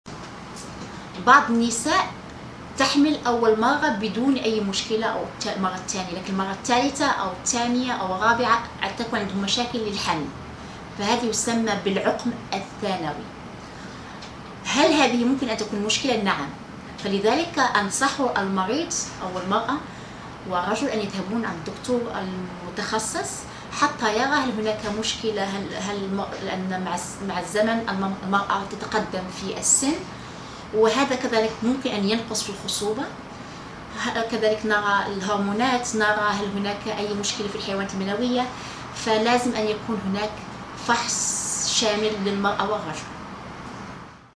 Secondary Infertility
Dr. Nadia Najjari, Specialist IVF at Fakih IVF Fertility Center talks briefly about Secondary Infertility